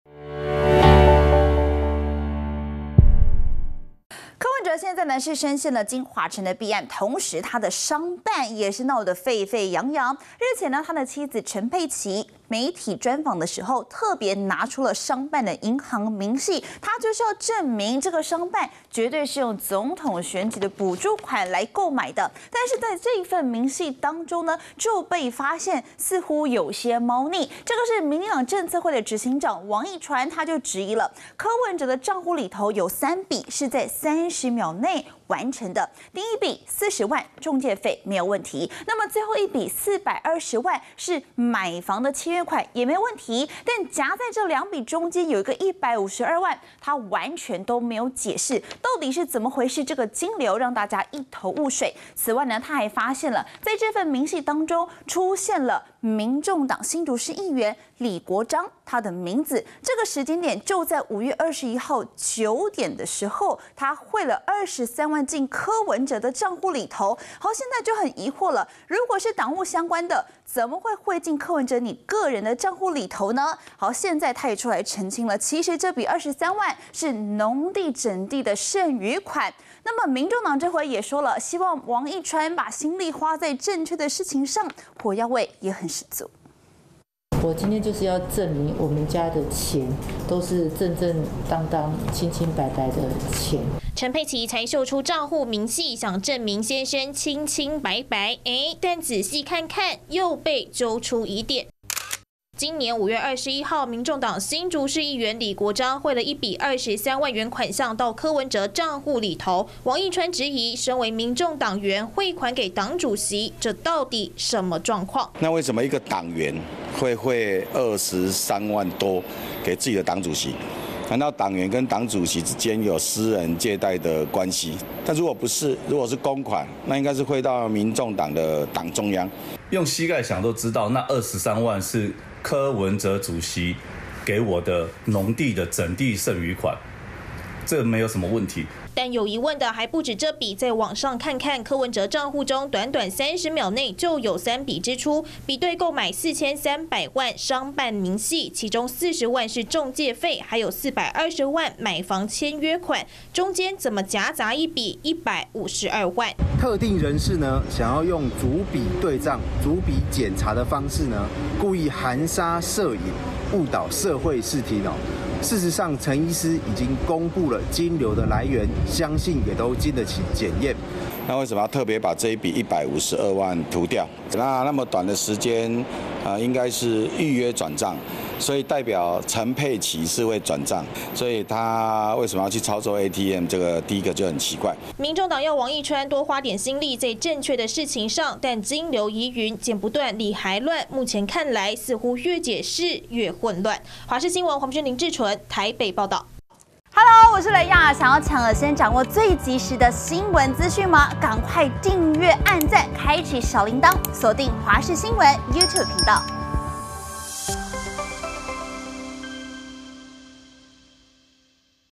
遭爆匯23萬進柯文哲帳戶 李國璋：農地整地剩餘款｜華視新聞 20240915
民眾黨主席柯文哲的太太陳佩琪，日前秀出帳戶明細，試圖證明柯文哲的金流沒有問題，但這些資料反而讓外界找出更多疑點，民進黨政策會執行長王義川發現，民眾黨新竹市議員李國璋，在今年5月21日，匯了23萬到柯文哲的私人帳戶，質疑難道黨員跟黨主席有借貸關係嗎？這筆款項用途又是什麼？另外，柯文哲買商辦30秒內的3筆匯款，其中一筆152萬似乎跟買房無關，面對爭議，李國璋回應，23萬是柯文哲給他的農地整地剩餘款，民眾黨則回嗆，請王義川花心力在正確的事情上。